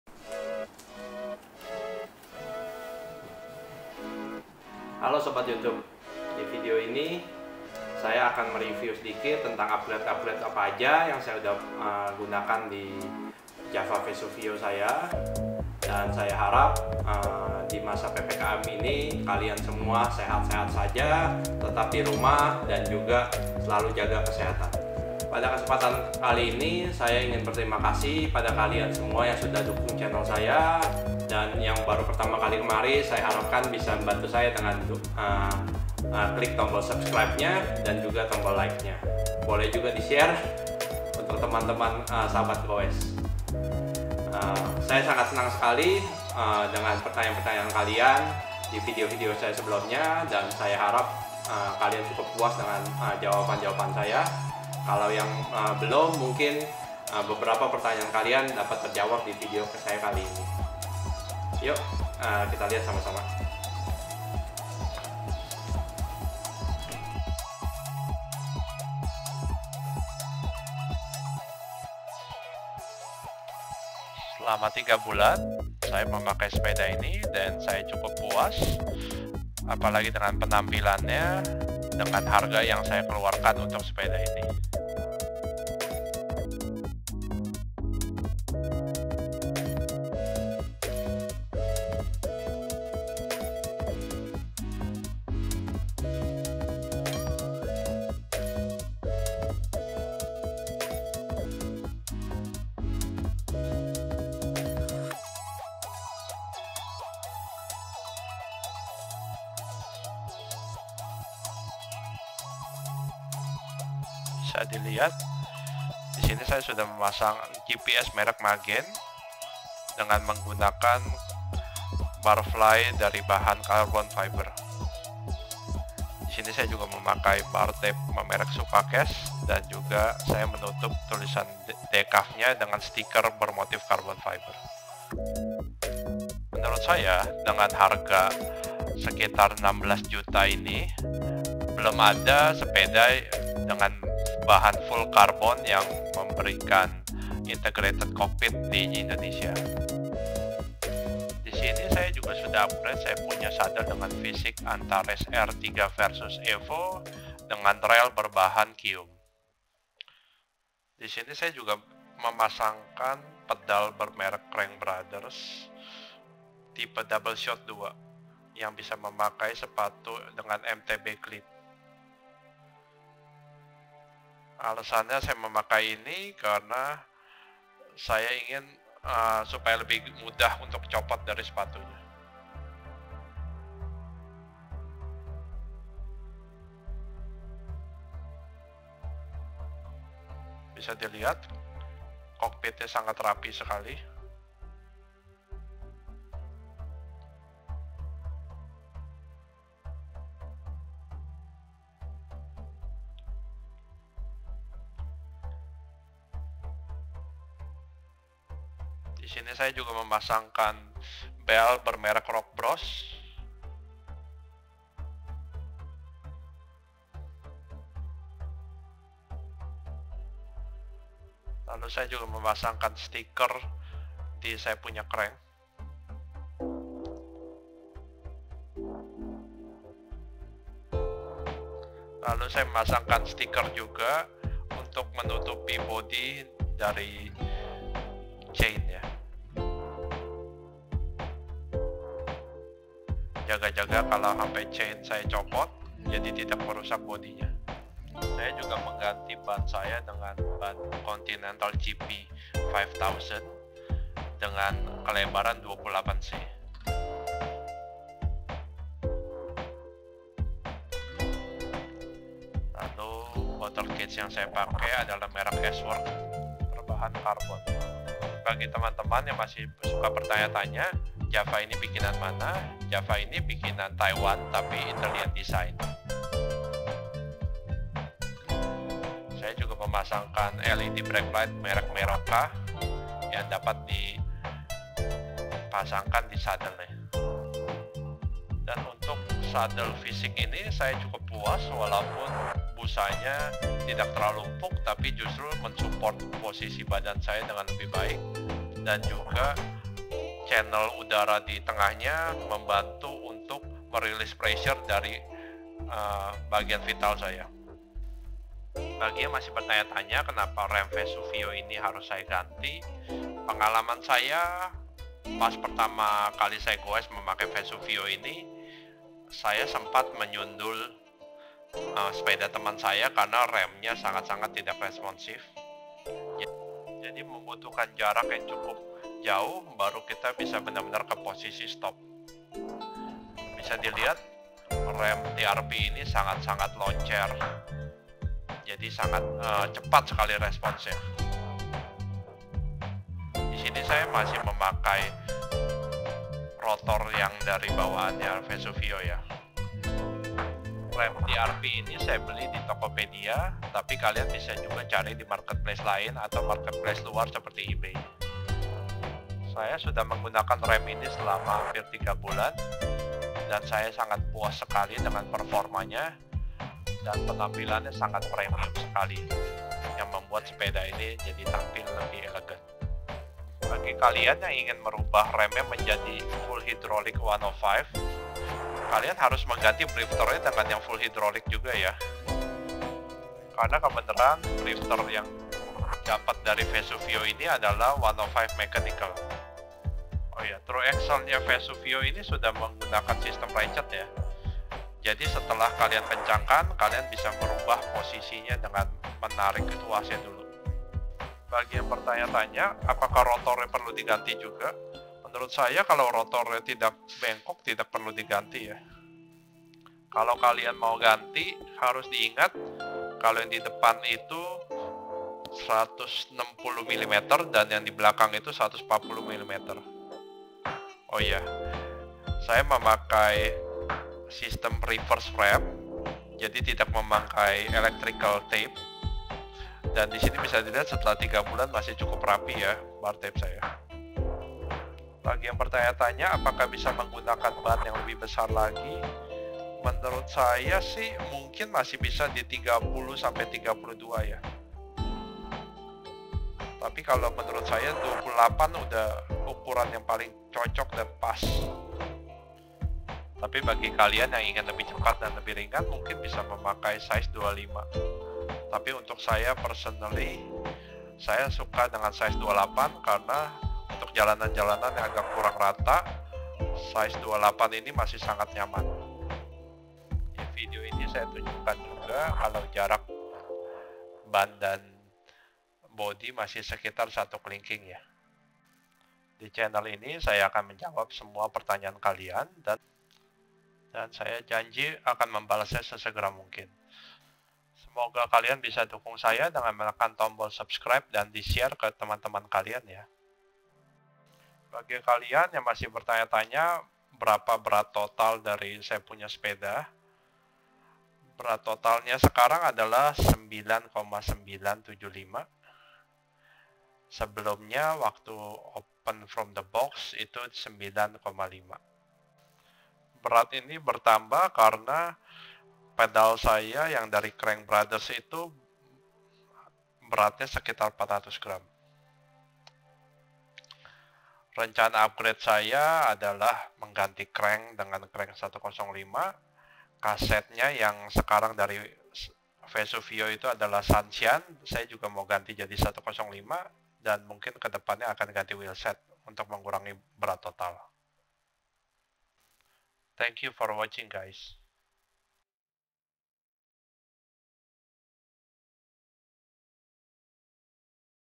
JAVA VESUVIO 2021 - AFTER 3 MONTHS UPGRADES SUMMARY + REVIEW - Q&A FROM PREVIOUS VIDEOS COMMENTS
Hi Riders, Wish You ALL save and well. Hope this video will answer most of your questions about the Java Vesuvio. I will answer them as soon as possible. Thank you very much.

Hallo Sahabat Gowes, Semoga kalian semua dalam keadaan sehat dan baik. Saya harap video ini dapat menjawab semua pertanyaan kalian tentang Java Vesuvio ini. Bila mana ada pertanyaan yang belum terjawab, silahkan tulis komen di bawah dan saya akan menjawabnya secepat mungkin. Terima kasih.